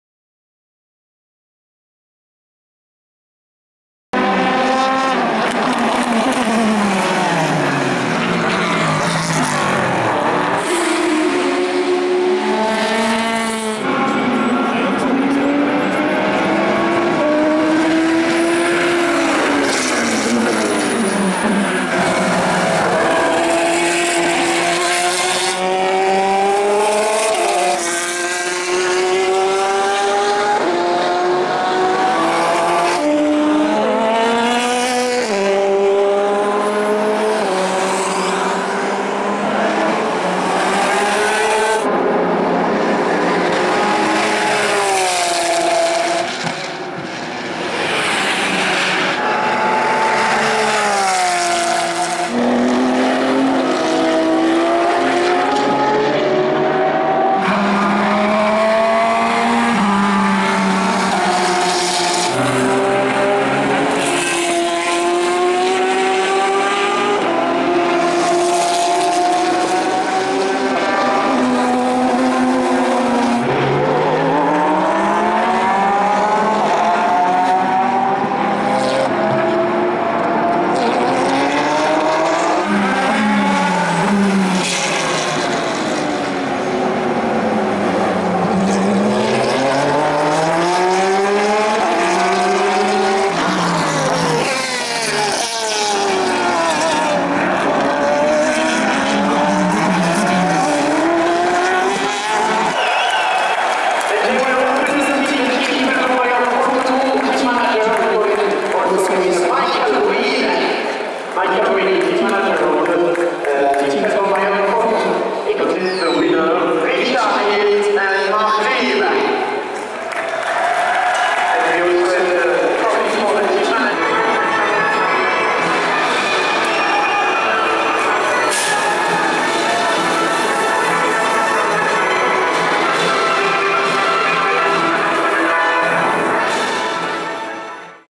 Felbermayr Proton Porsche 997 RSR WEC Spa Francorchamps 2012
The German Felbermayr Proton Team denies this Year the World Endurance Championship WEC wit two Porsche 997 RSR. One at the GTE-Pro with Marc Lieb and Richard Lietz and one at the GTE-AM. The team was able to move into the season a couple of victories, such as here at Spa Francorchamps. The video also shows the award ceremony of the GTE-Pro.

RF 1993 Sportscars Racing Videos